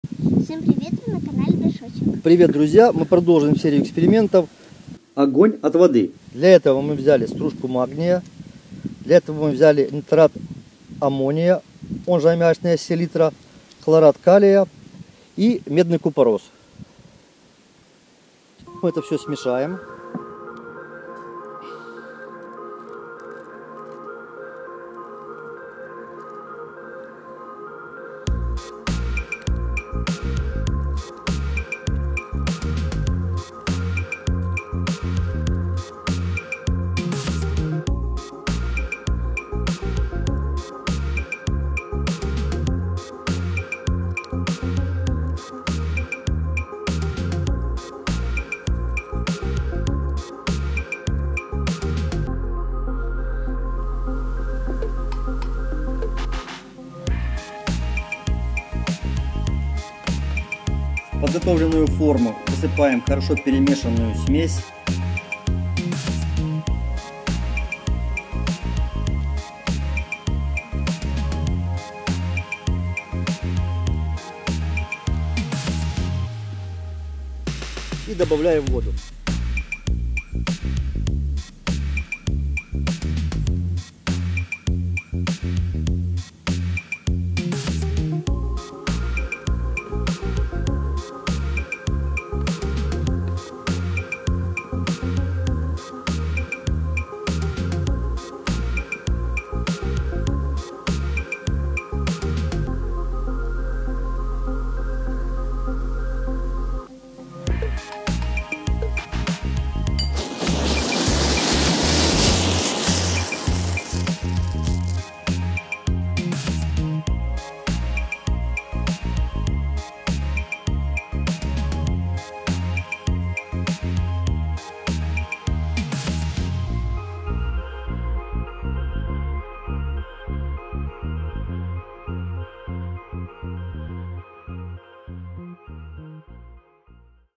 Хлорат калия + магний + нитрат аммония + медный купорос. Огонь от воды.
Данное видео демонстрирует один из простейших, но красивых химических экспериментов воспламенения химических элементов от воды.
Внимание! Предупреждаем, что химические эксперименты проводят ТОЛЬКО взрослые!